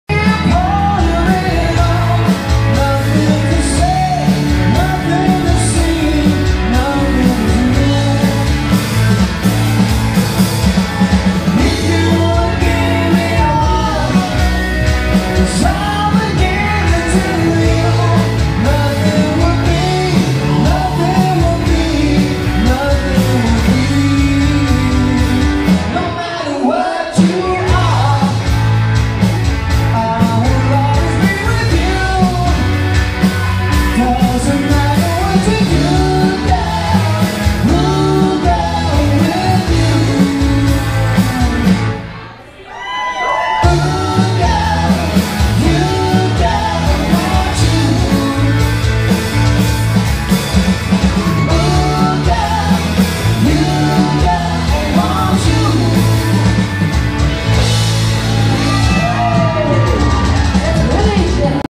Willie Jeffries on vocals with Krush - 2016
John Martin - Bass
Greg Hood - Keys
Michelle Metzger - Vocals
Johnny Anderson - Guitars
Casey Banta - Drums
Willie Jeffries - Vocals